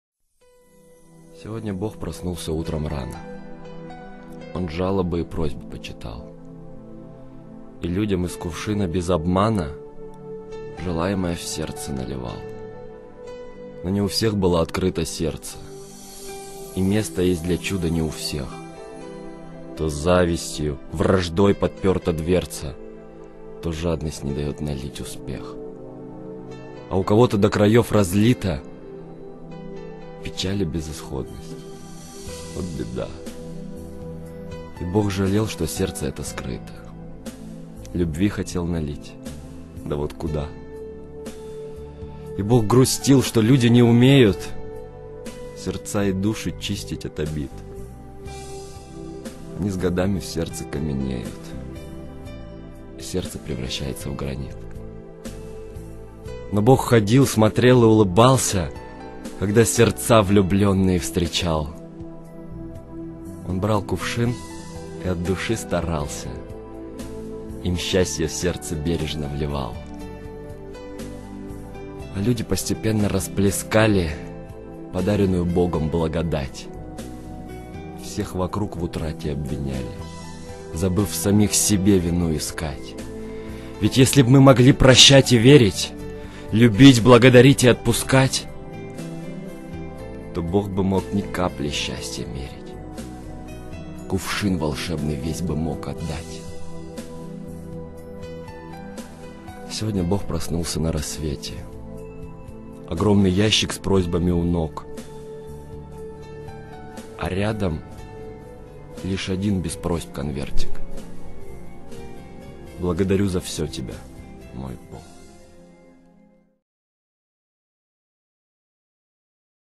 Сегодня Бог, проснулся...
Автор: И. Самарина
Читает текст: М. Калужский